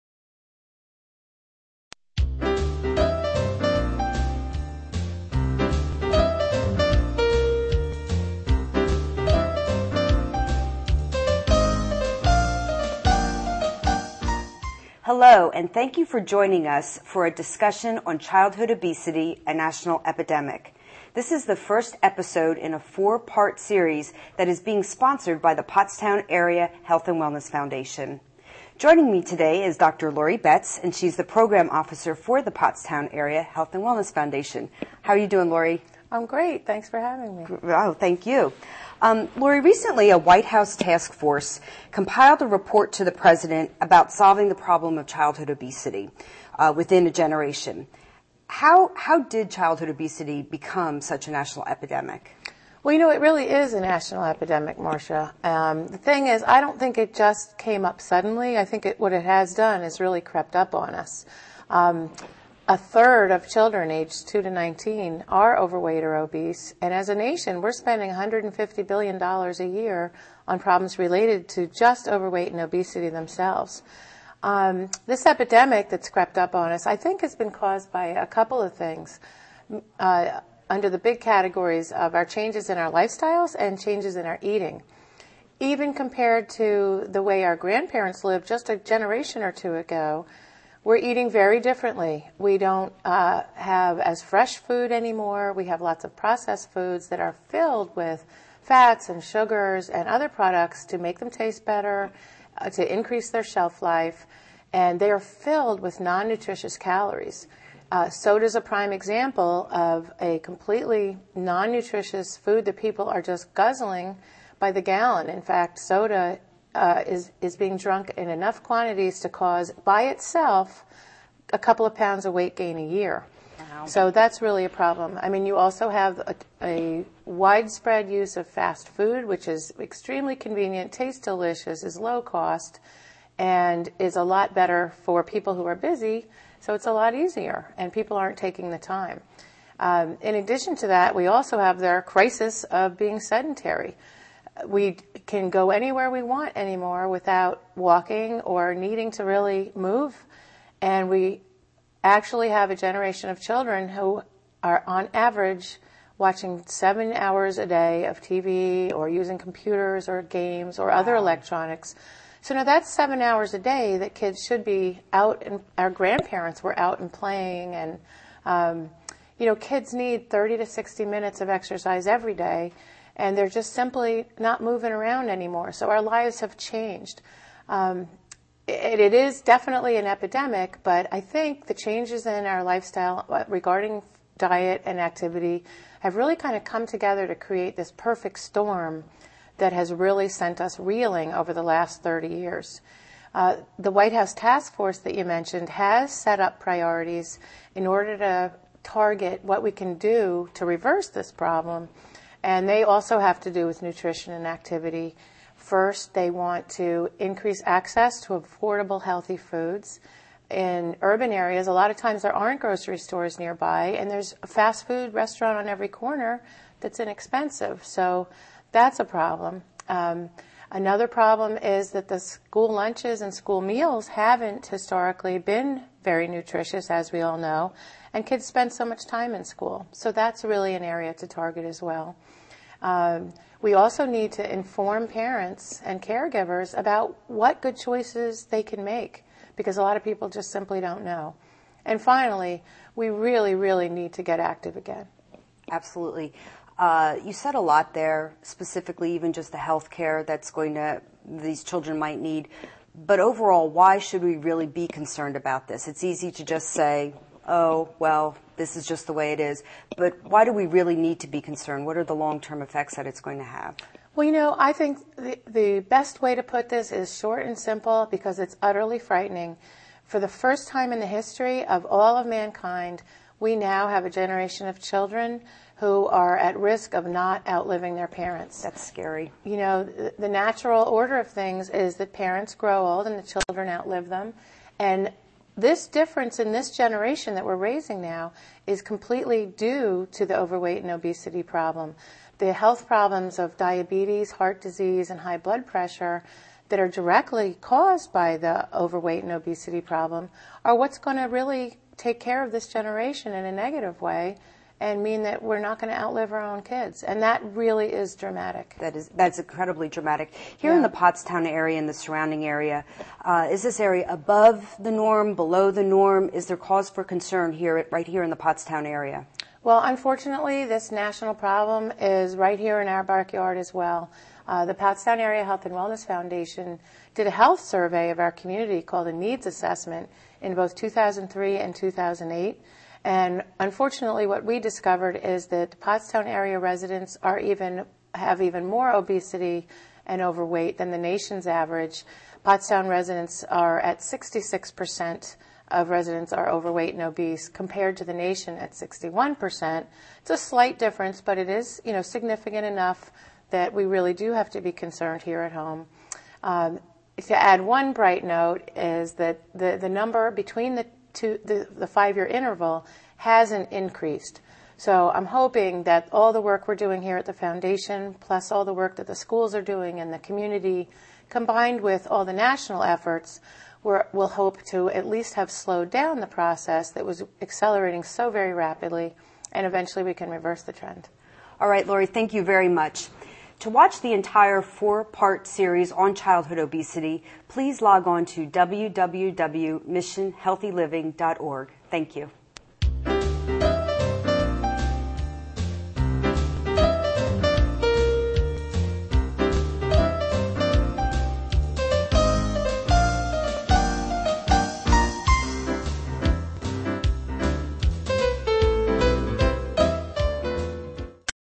Child Obesity Part 1 – A National Epedemic
Dr. Laurie Betts, Program Officer at the Pottstown Area Health & Wellness Foundation talks about the national epidemic.

Pottstown Health and Wellness Foundation's Mission Healthy Living Webisode Series.